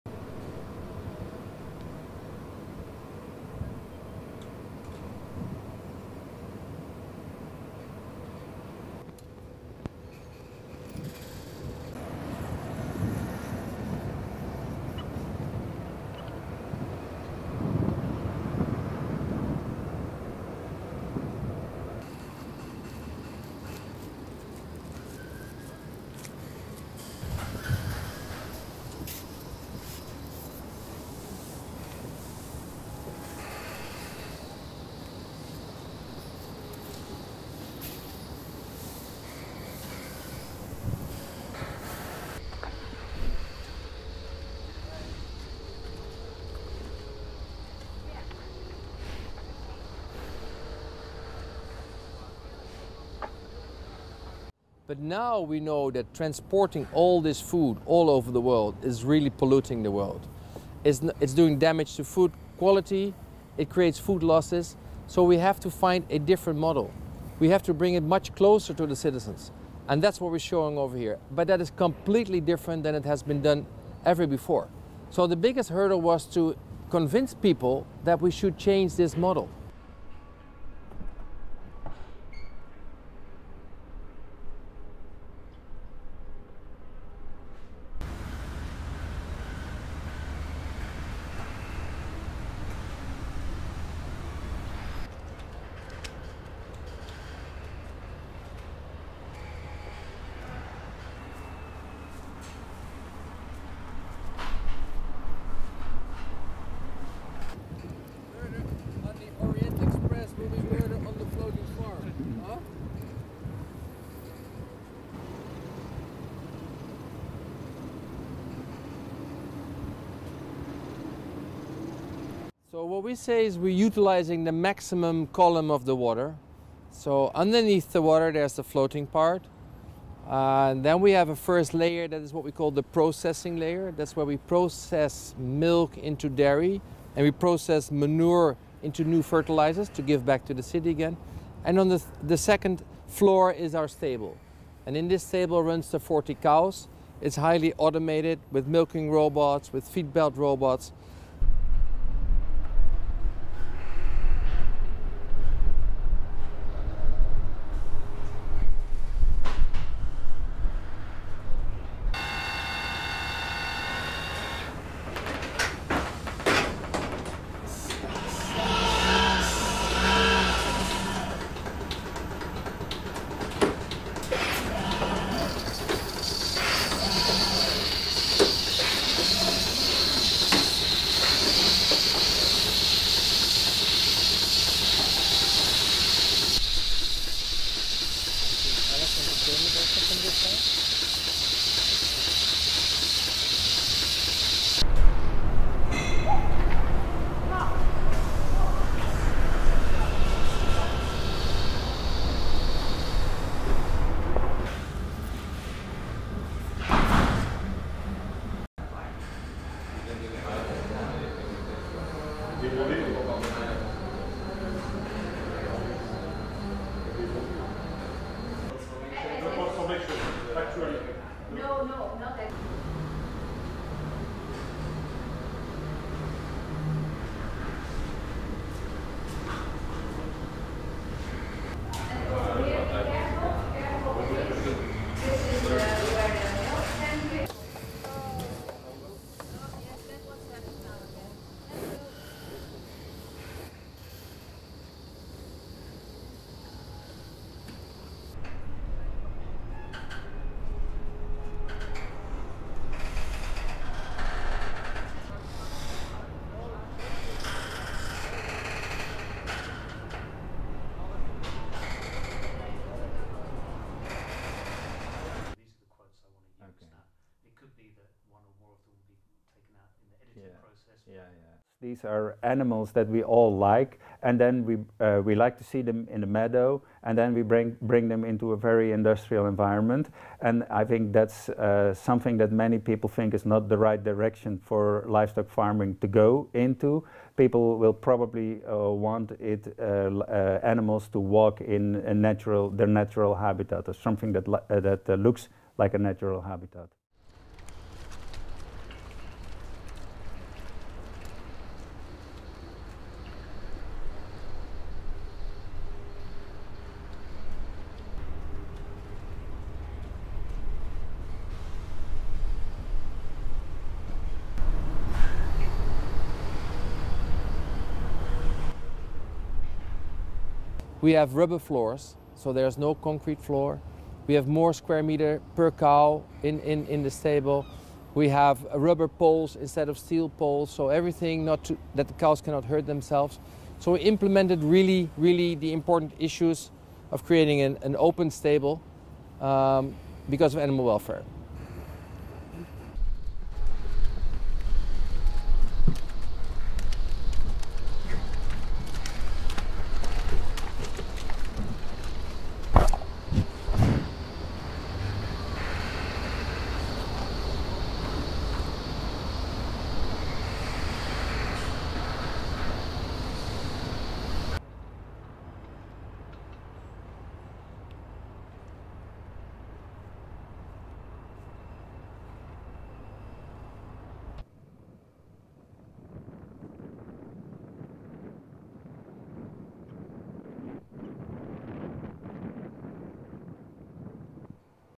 Floating dairy houses cows in the Netherlands
Food security and rising sea levels are both future global concerns, as large areas of agricultural land may be lost under the water due to climate change.
One engineer turned farmer in the Netherlands believes he has found a solution. float farms on water.

STORY-LINE :
This floating farm dairy smallholding smells just like any other farm - the pungent aroma of cow manure hangs in the air long before the unusual stable housing the cattle is visible.
The scent may be the same; the farm itself is far from familiar.
Moored in an offshoot of Rotterdam's busy port, the farm is a futuristic three-story floating structure where one robot milks the cows and another automatically scoops up the manure that gives the enterprise its familiar smell.
Its roof collects rainwater and a raft of solar panels floating alongside produces 40 percent of the energy the farm needs.
The cows eat a mixture of grass cut from a local golf course and the pitch used by Rotterdam's top soccer team, grain used by a local brewer to make beer and potato peelings - all automatically cut, mixed and transported to food troughs by conveyer belts.
The fully-functioning showcase of circular-economy farming combines Dutch expertise in recycling, building on water and automated agriculture.
Van Wingerden, an engineer, believes the farm, which opened in May and cost about 3 million euros ($3.4 million), demonstrates a new way of producing food, close to where most of it is consumed - in the world's cities, thus reducing carbon emissions caused by transporting food long distances.
"Transporting all this food all over the world is really polluting the world. It's doing damage to food quality, it creates food losses," he says. "So we have to find a different model. We have to bring it much closer to the citizens. And that's what we're showing over here."
The project is drawing interest from around the world. Van Wingerden says he is already discussing floating farms in Singapore, China and New York.
When the herd reaches its target capacity of 40 cows (there are currently 35), it will produce 800 liters (211 gallons) of milk each day. The brown and white cows are a breed called Maas-Rijn-Ijssel - named for three rivers that flow through the Dutch region they originate from.
The farm sells the milk and yogurt that is pasteurized and produced on the middle floor of the pontoon to local restaurants and through supermarkets in Rotterdam. People also can buy unpasteurized milk straight from a tap at the farm. Manure is processed for use as fertilizer.
Jan Willem van der Schans, a senior researcher at Wageningen Economic Research who specializes in urban farming and circular economy issues, says floating farms could be the future for some sectors of agriculture such as fruit and some vegetables in some parts of the world. But he thinks that the level of automation and the unnatural surroundings of the cows may create opposition to the project.
"These are animals that we all like and then we like to see them in a meadow," he says. "And then we bring them into a very industrial environment and I think that's something that many people think is not the right direction for livestock farming to go into."
Van Wingerden says that animal welfare is his top priority, pointing to many design elements in the construction that are intended to make life as easy as possible for the cows such as rubber floors and poles in the stable.
The pontoon rises and falls gently on undercurrents caused by the movement of nearby ships.
If cows get seasick, these weren't showing any signs.